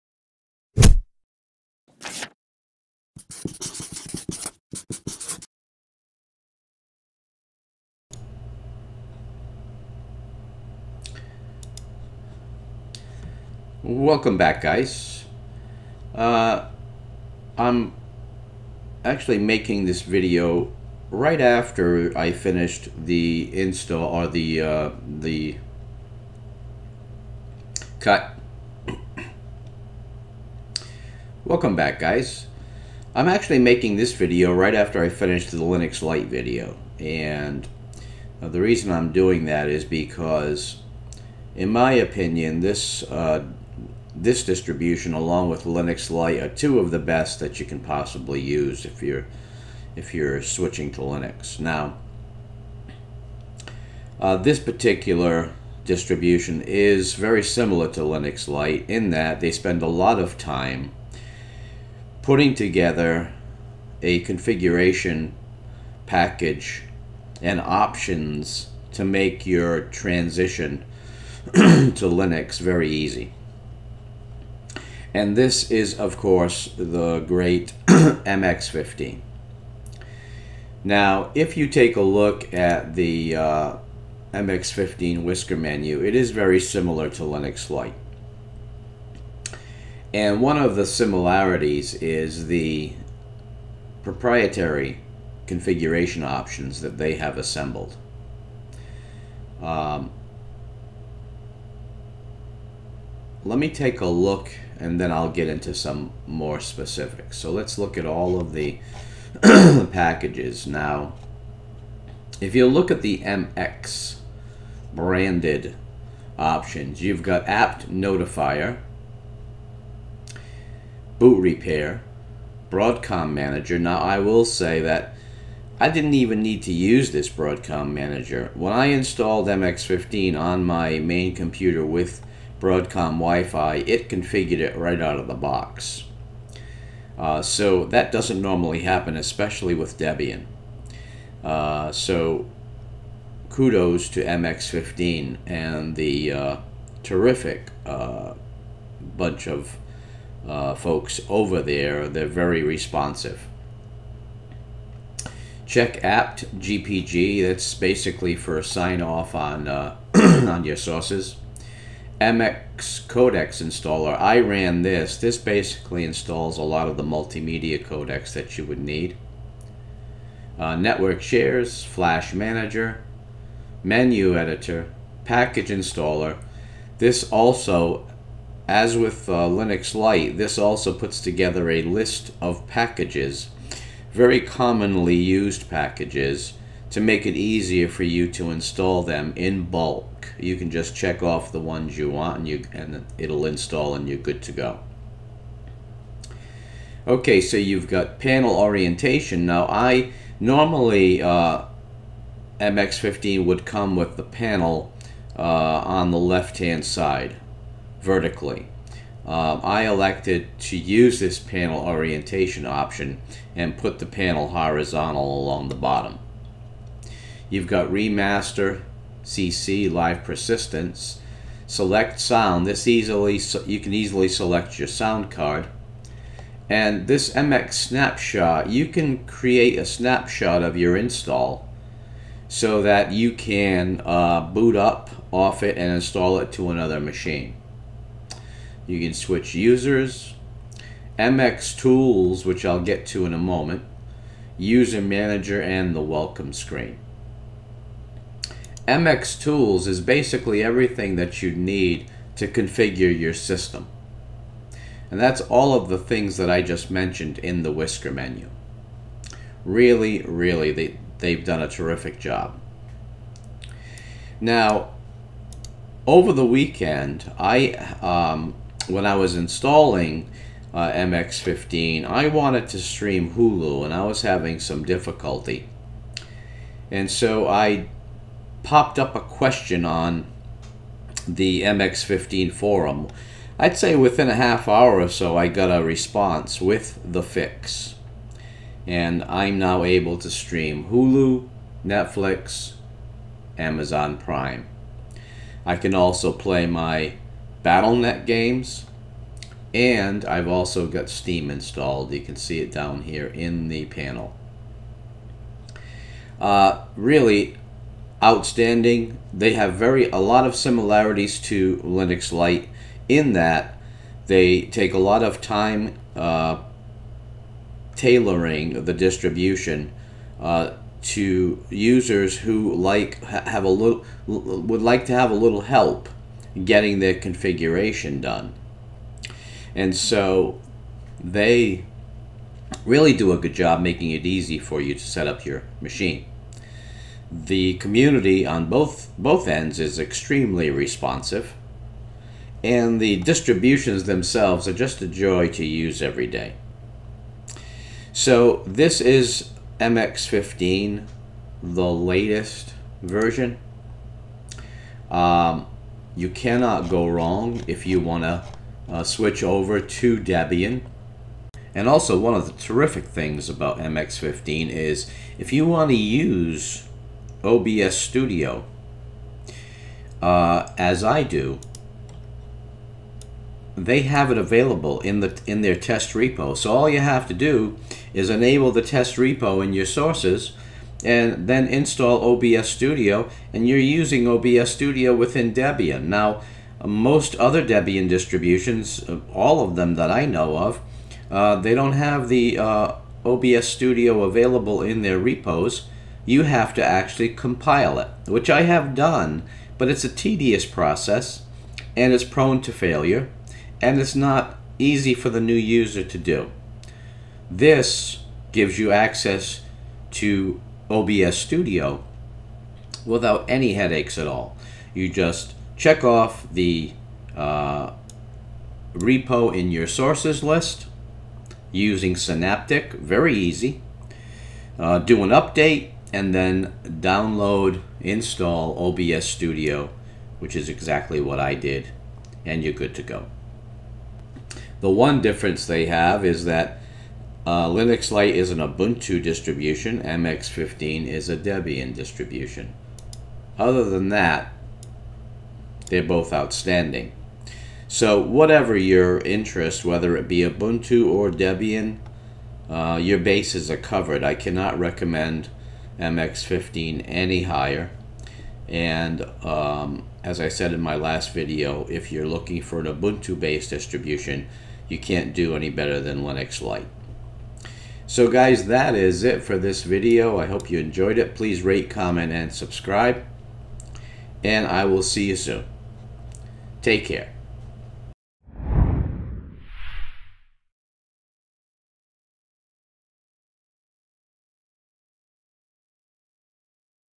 MX-15 Linux - Debian at it's Finest!
In this video I take another look at a great Debian distro...MX-15. Hope you enjoy!